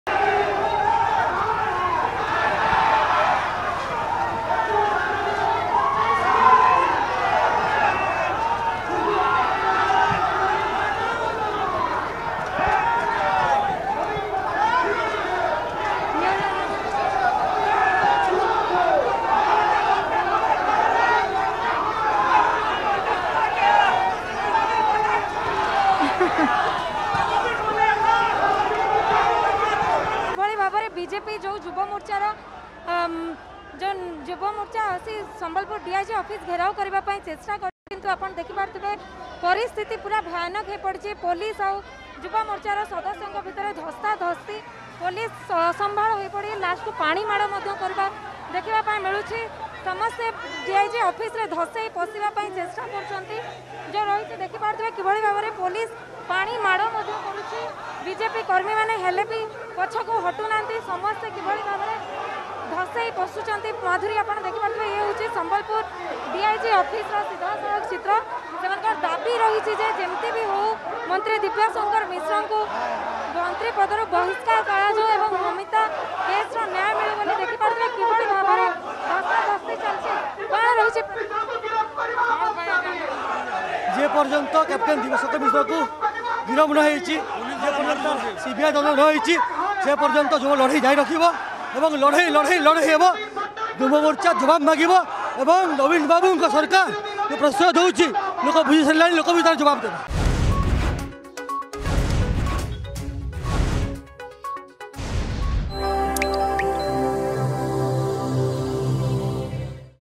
ବିଜେପି କର୍ମୀଙ୍କ ଉପରକୁ ପାଣିମାଡ଼
ସମ୍ବଲପୁର ଡିଆଇଜି ଅଫିସ ଘେରାଉ ବେଳେ ବିଜେପି କର୍ମୀଙ୍କ ଉପରକୁ ପାଣିମାଡ଼
ମମିତା ମେହେର ହତ୍ୟା ମାମଲା ସିବିଆଇ ତଦନ୍ତ ଓ ମନ୍ତ୍ରୀ ଦିବ୍ୟଶଙ୍କର ମିଶ୍ରଙ୍କ ବହିଷ୍କାର ଦାବିରେ ବିଜେପି ପକ୍ଷରୁ ସମ୍ବଲପୁର ଡିଆଇଜି ଅଫିସ ଘେରାଉ କରାଯାଇଛି ।